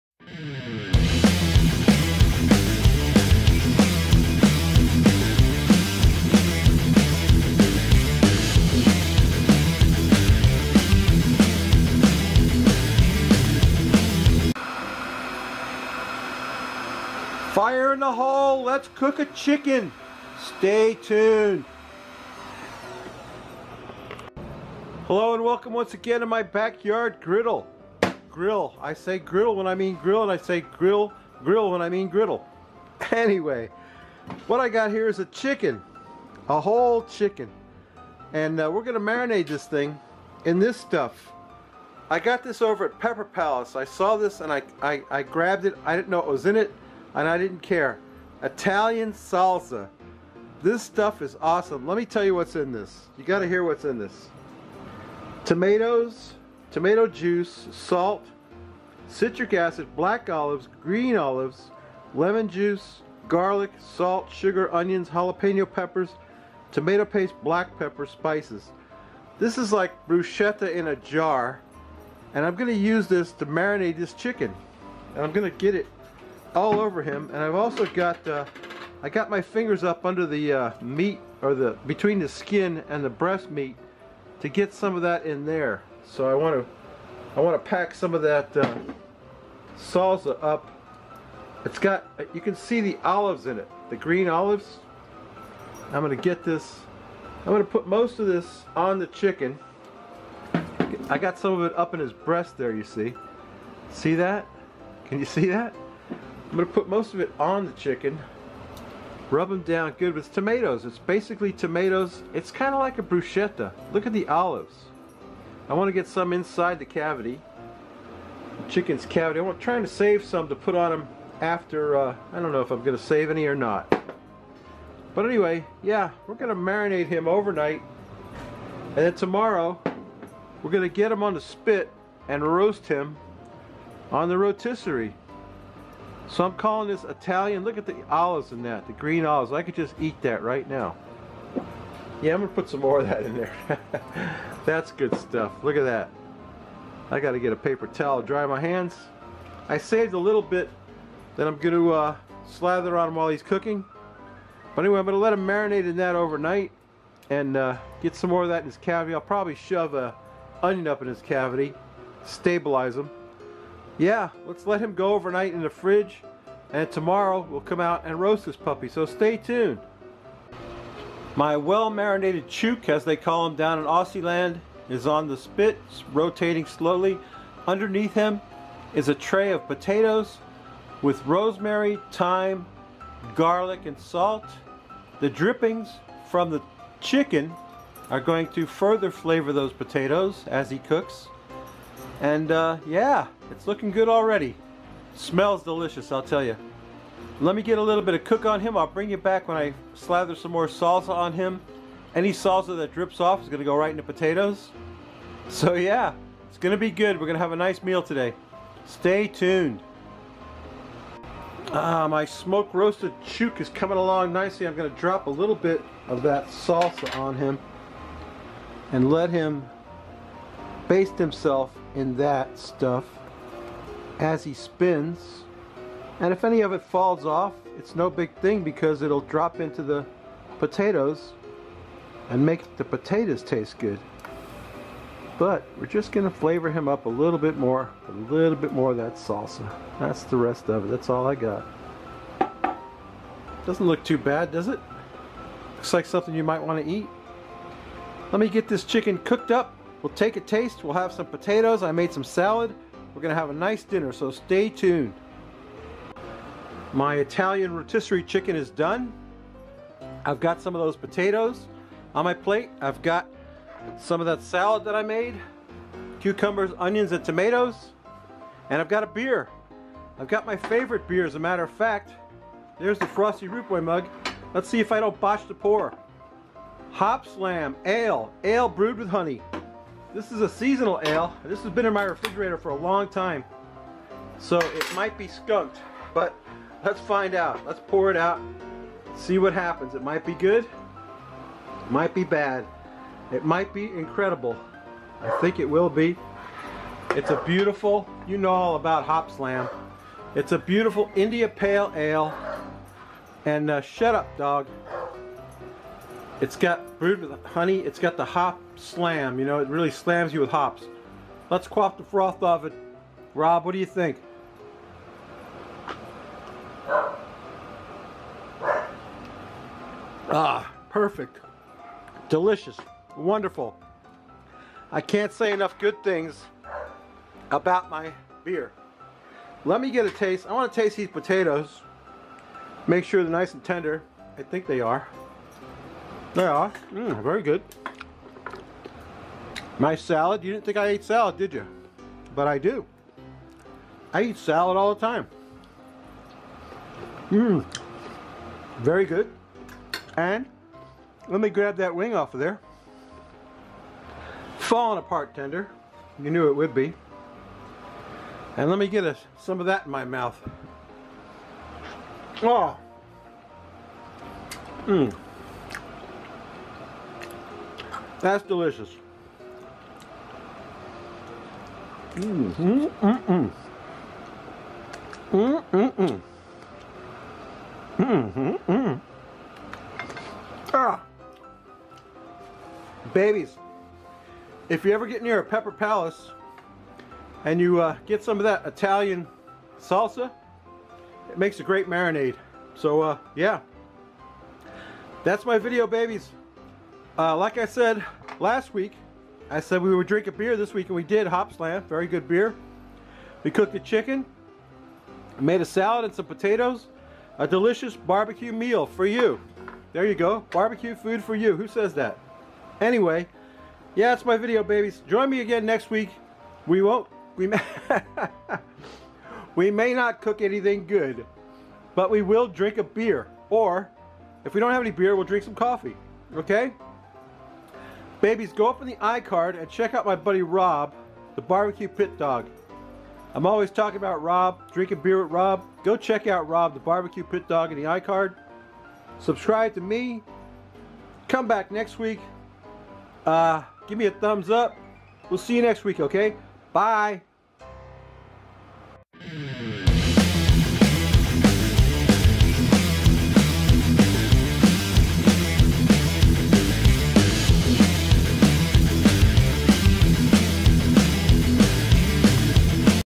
Italian Rotisserie Chicken
I went over to my favorite store, Pepper Palace, and picked up this jar of Italian Salsa! The rest, as they say, is history!